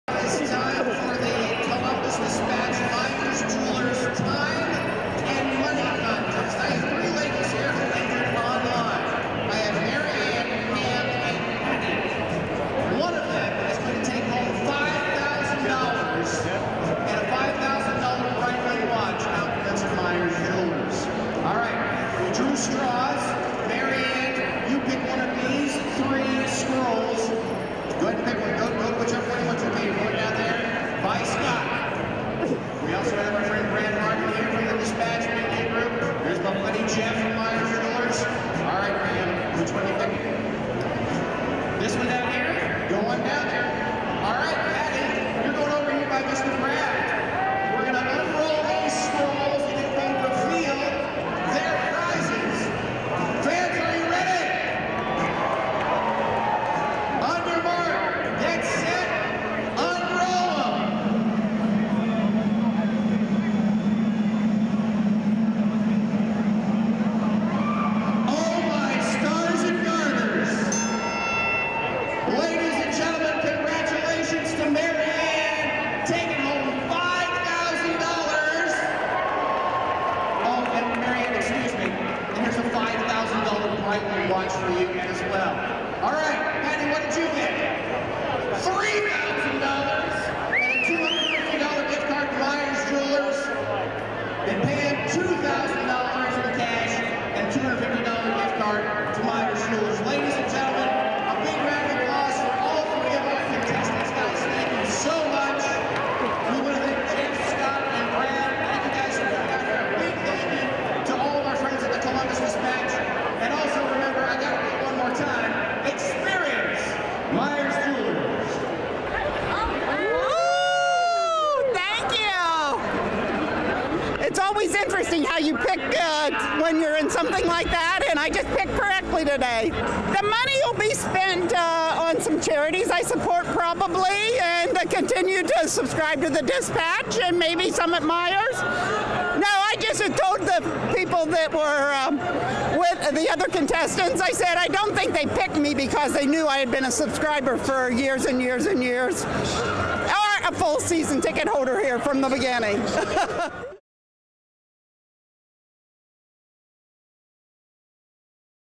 Blue Jackets | Hilliard woman wins cash, $5,000 watch
Columbus Dispatch, Fred Meyer Jewelers promotion held during hockey intermission